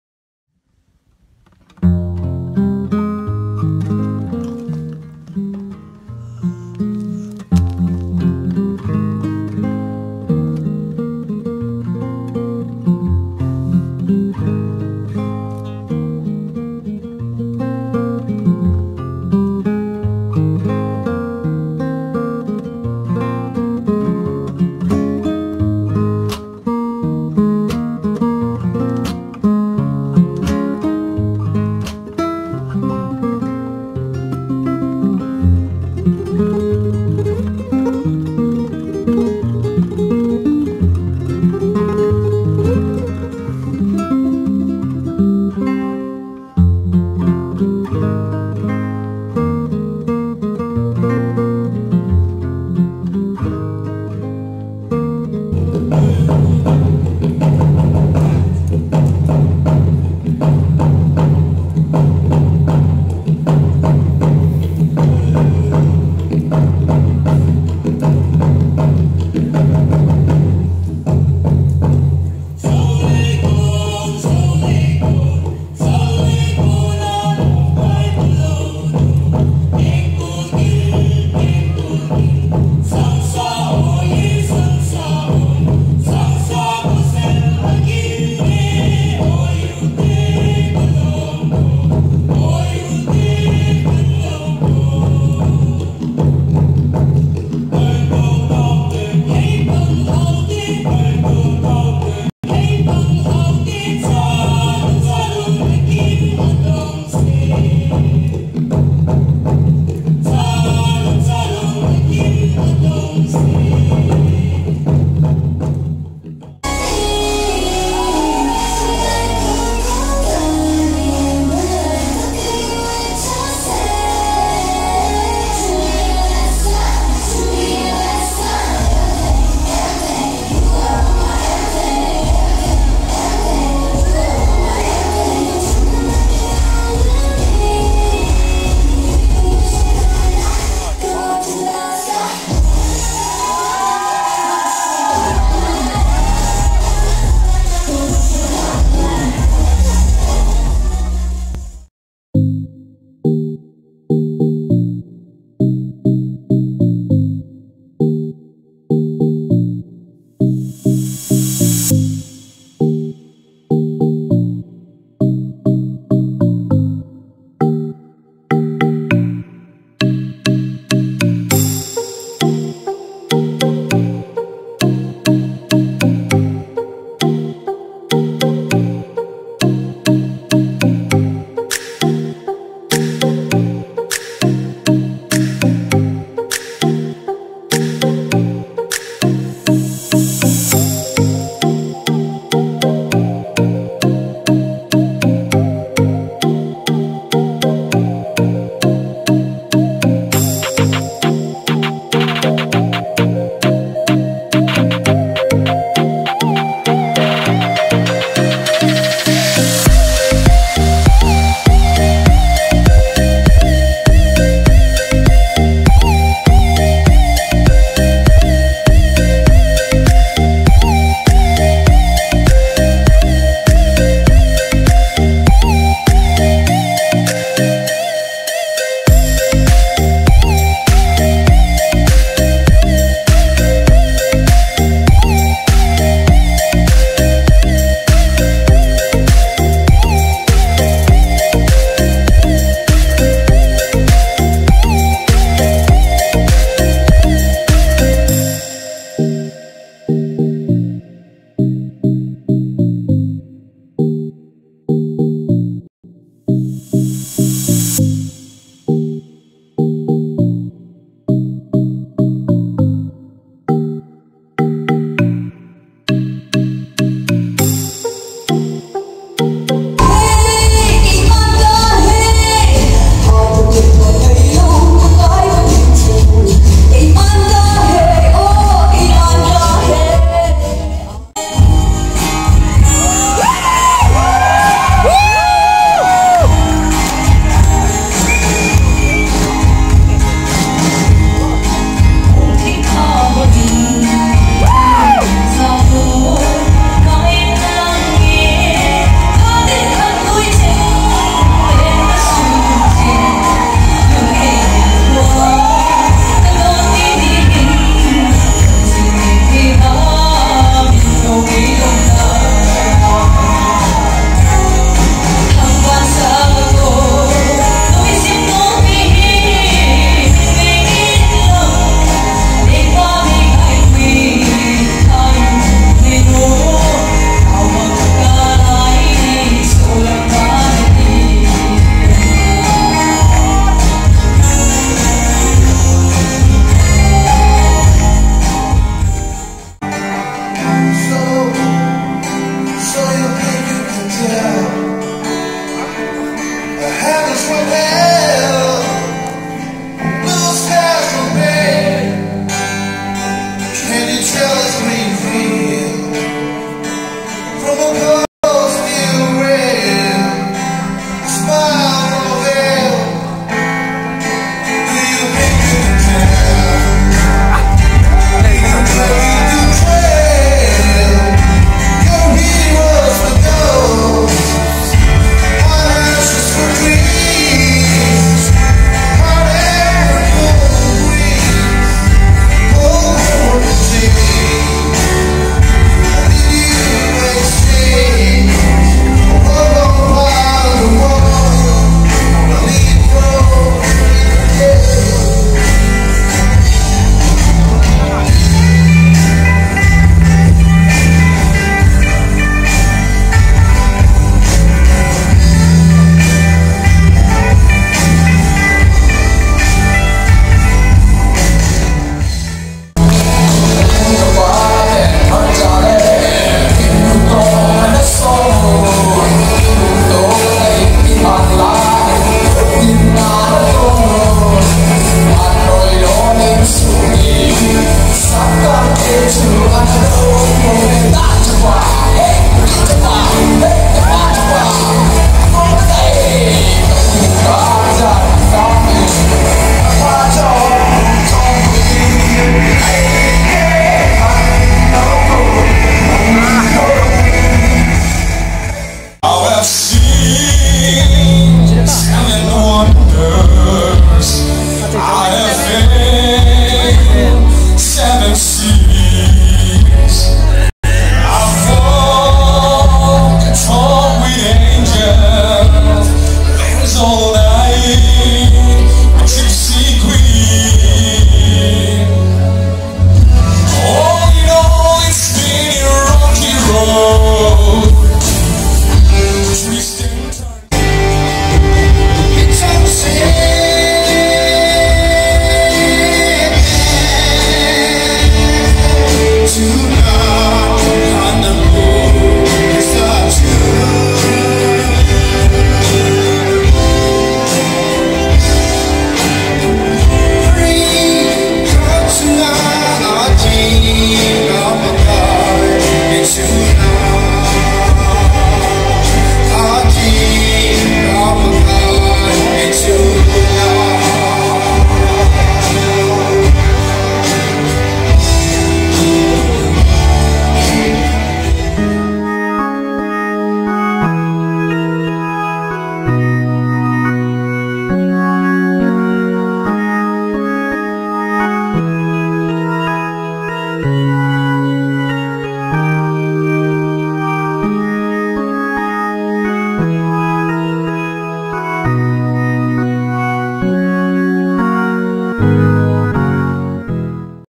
My First Freshers' Social of KSOD
Lately, I found myself developing a phobia for exposure to big gatherings. So I took this adventure to chide myself rather than just hide. Hats off to all involve to realise this social function!